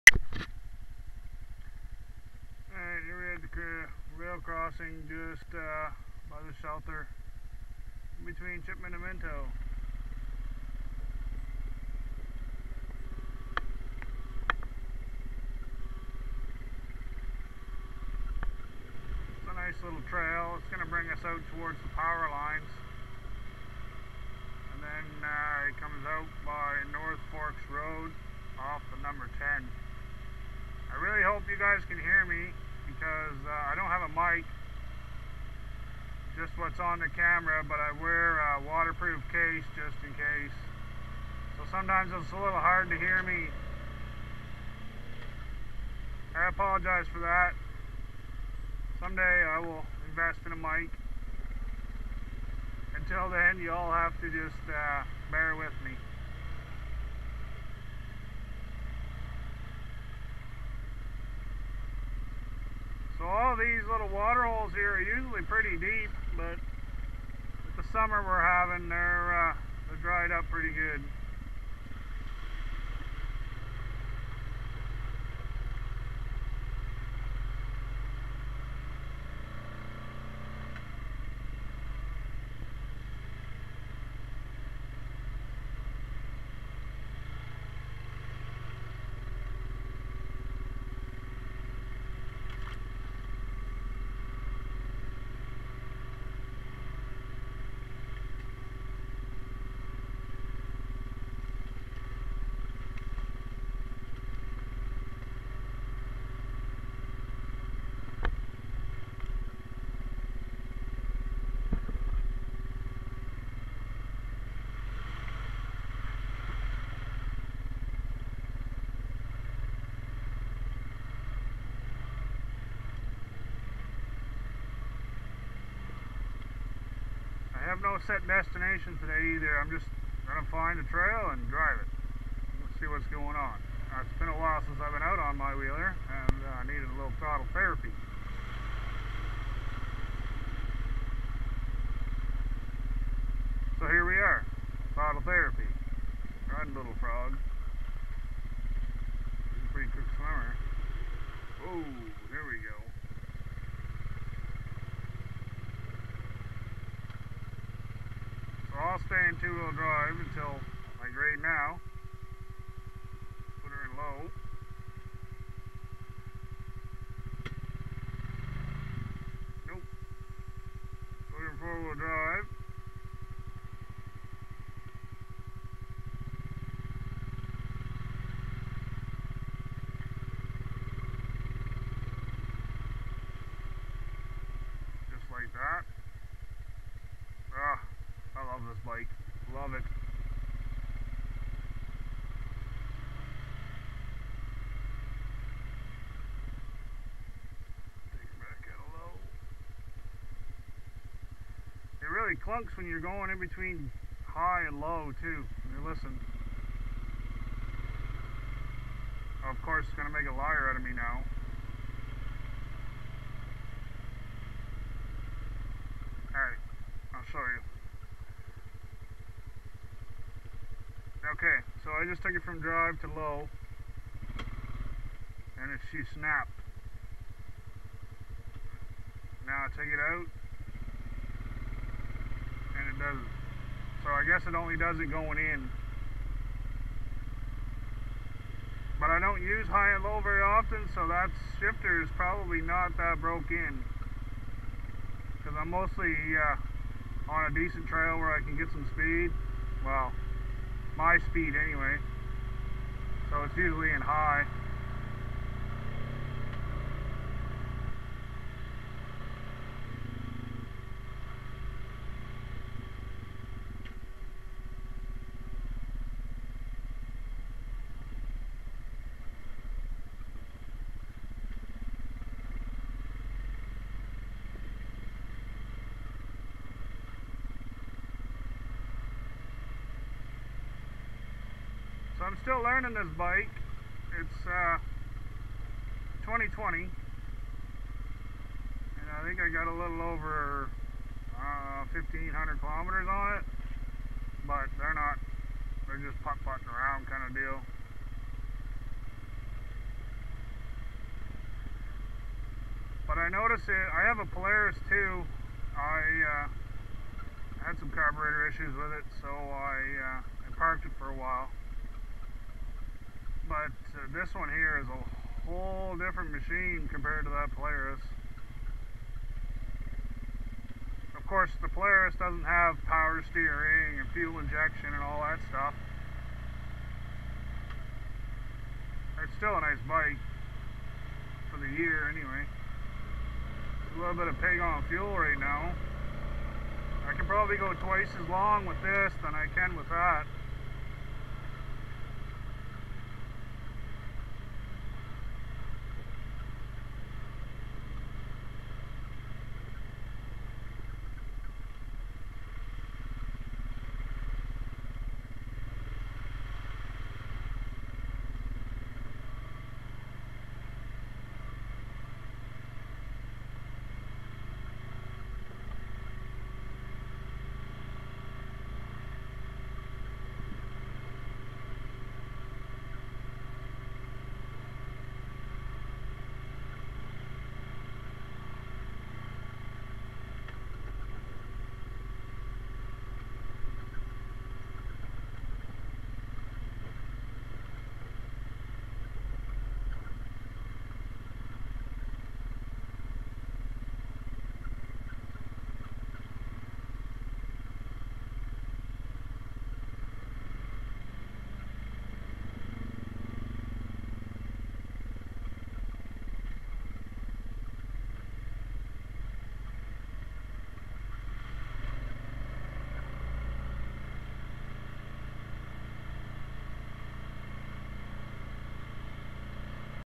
Dried up pretty good (Honda Rubicon 520 )
Hi in this video I'm leaving a snow shelter between Chipman and Minto NB. I like this trail but it has some juicy water/muddy spots .This year has been pretty dry so its not so juicy anymore. this trail will come out to the power lines and also to route 10 by north forks road.